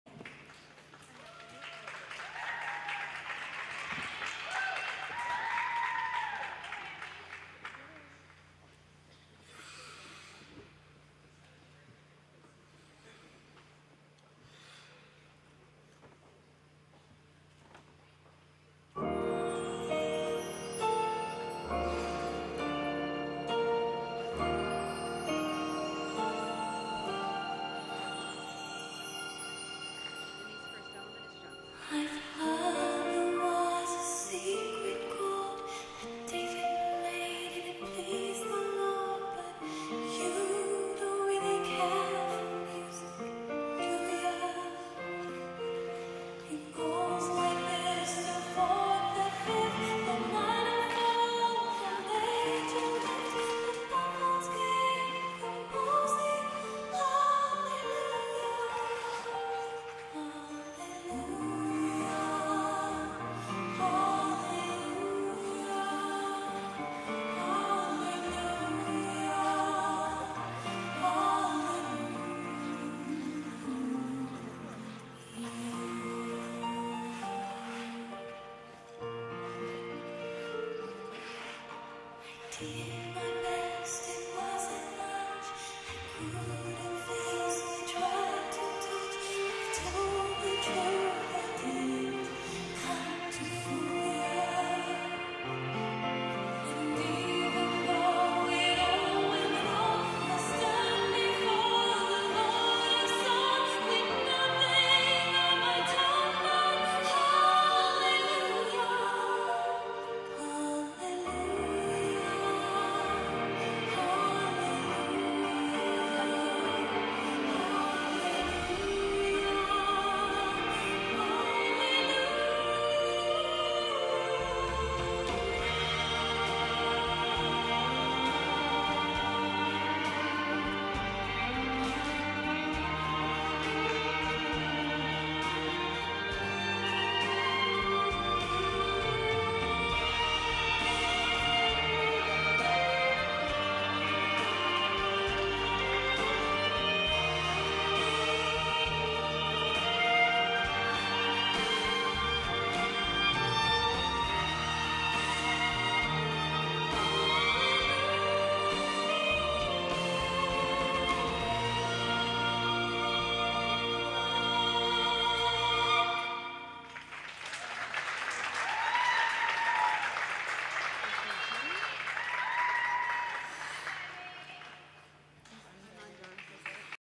Emmy Ma - '15 New England Regionals - Novice Short Program
This is Emmy Ma's novice short program which was performed at the 2015 New England Regionals. She earned 41.62 points and placed second in the segment.